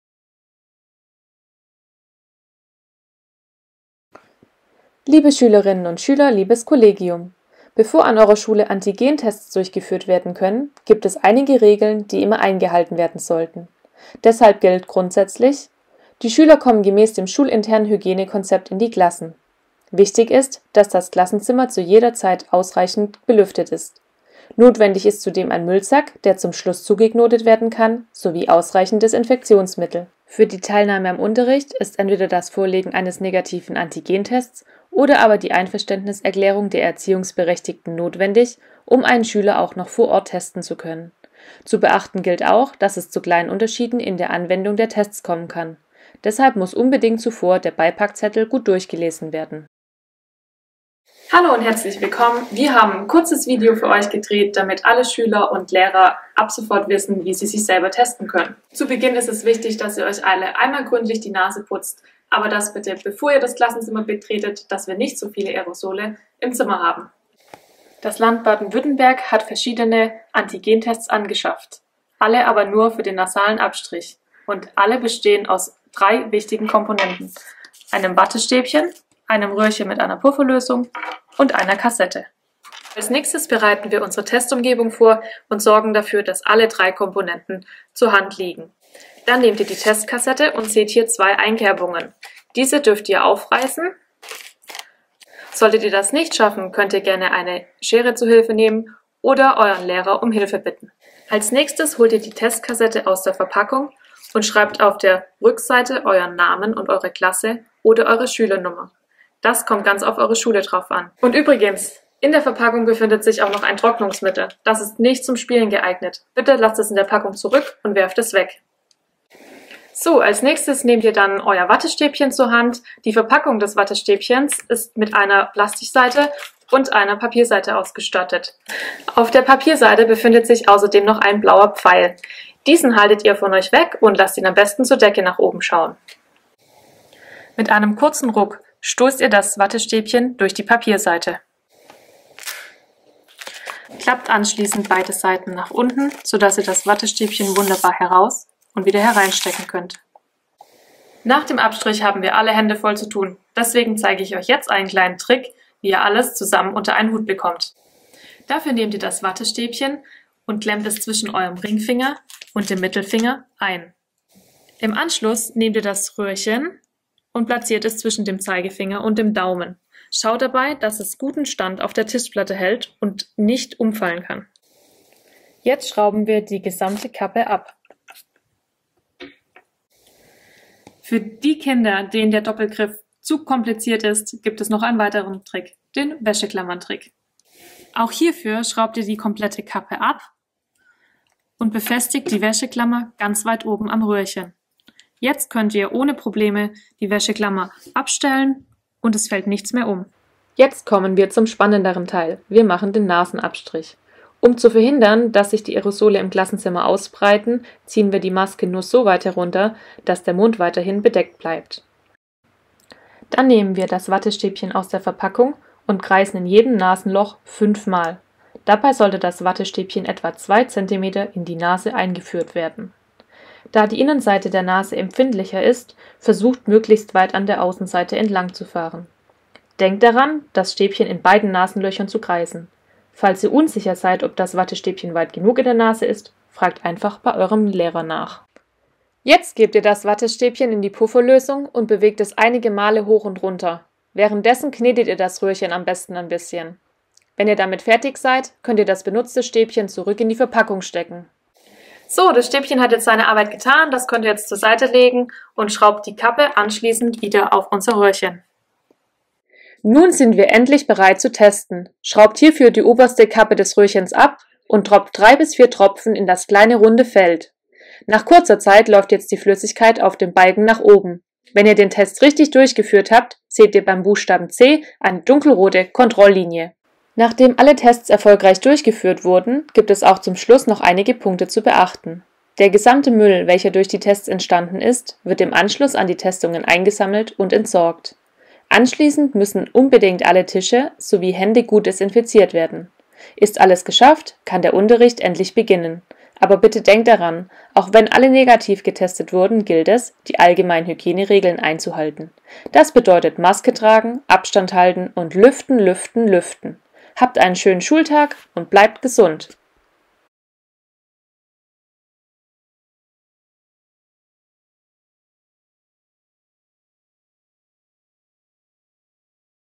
Schulungsvideo COVID-19 Antigen Selbsttest - Schülerinnen und Schüler | So gehts richtig | Apofaktur
In unserem Erklärvideo erklären wir dir detailliert, wie du ganz einfach & korrekt den Antigen Selbsttest durchführen kannst und das Schritt für Schritt.